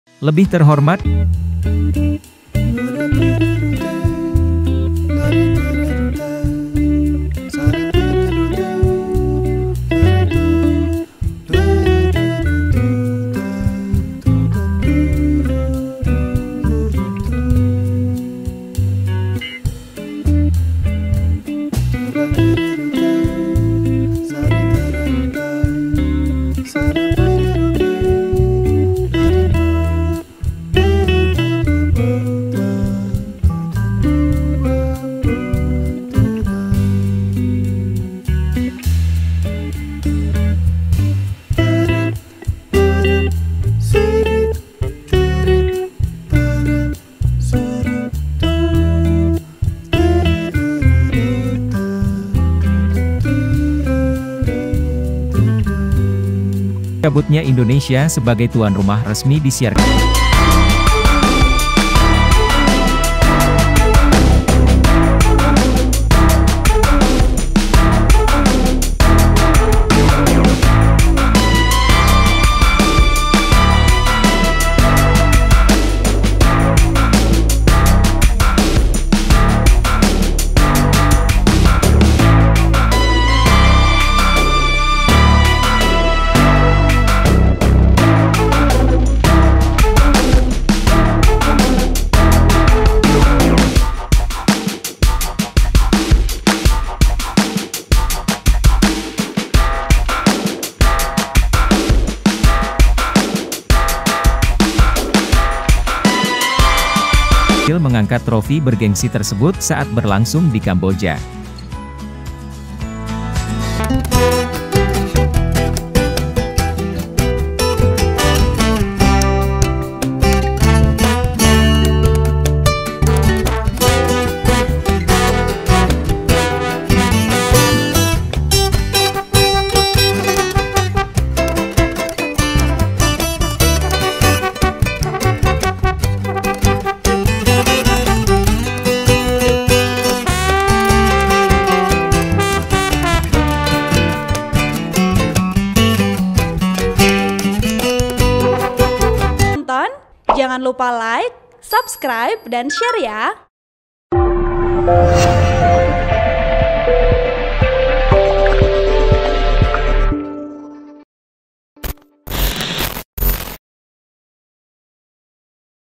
Lebih Terhormat dari Timnas Indonesia, Vietnam Undur Diri Tuan Rumah Piala AFF U23
POS-KUPANG.COM, - Ditengah keramaian hak Timnas Indonesia dicabut sebagai tuan rumah Piala Dunia U20, Vietnam bernasib lebih terhormat.

Vietnam mengundurkan diri sebagai tuan rumah Piala AFF U23 2023.

Dengan demikian hanya menyisakan Thailand sebagai calon tuan rumah Piala AFF U23.

Nasib Vietnam lebih terhormat ketimbang Indonesia. Minggu, 02 April 2023 (POS-KUPANG.COM, Arif Tio Buqi Abdulah)

Artikel ini telah tayang di Tribunnews.com dengan judul Lebih Terhormat dari Timnas Indonesia, Vietnam Undur Diri Tuan Rumah Piala AFF U23,

Penulis  : Bayu Satriyo Panegak
Editor  : Arif Tio Buqi Abdulah
VPO  : IRLAND